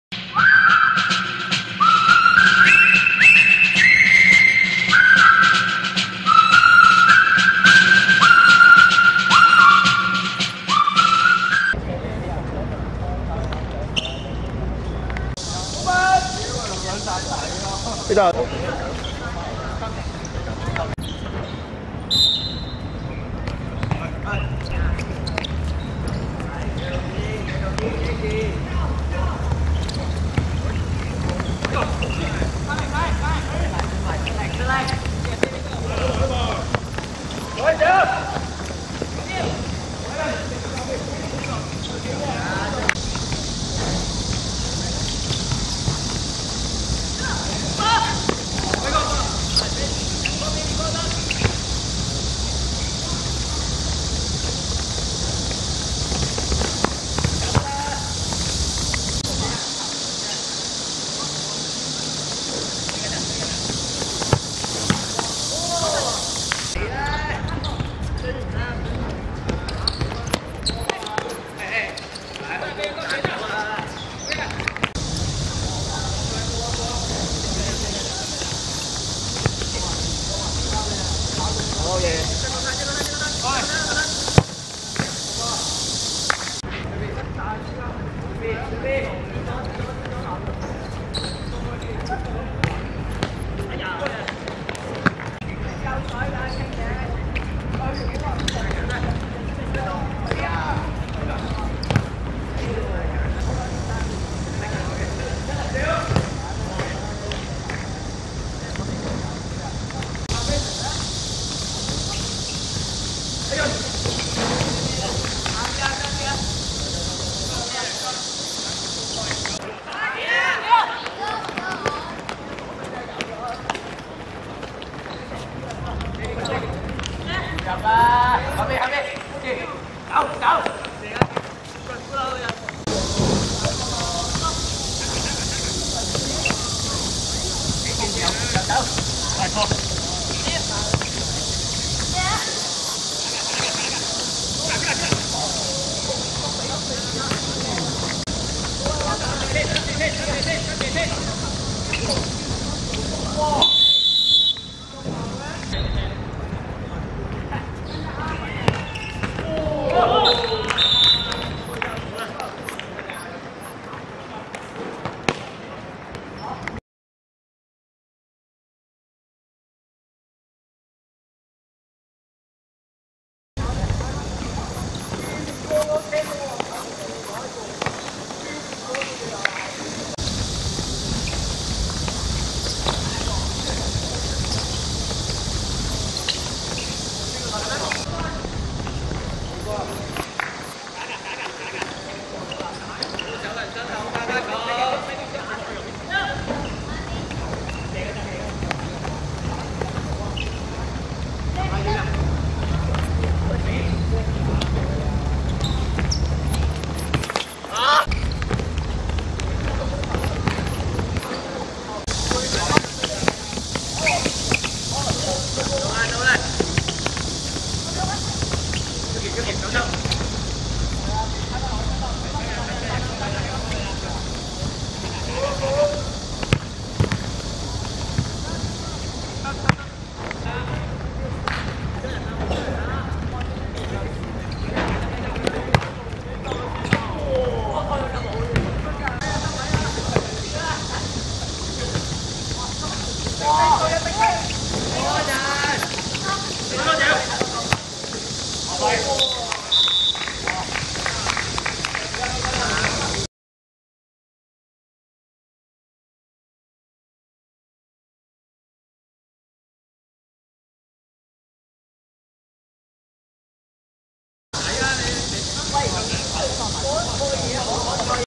侯寶垣 vs YHKFA (2017.6.21.草根足球同心盃分組初賽)精華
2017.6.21.天水圍公園五人足球塲 20:20
侯寶垣(白) 2 : 0 YHKFA (藍)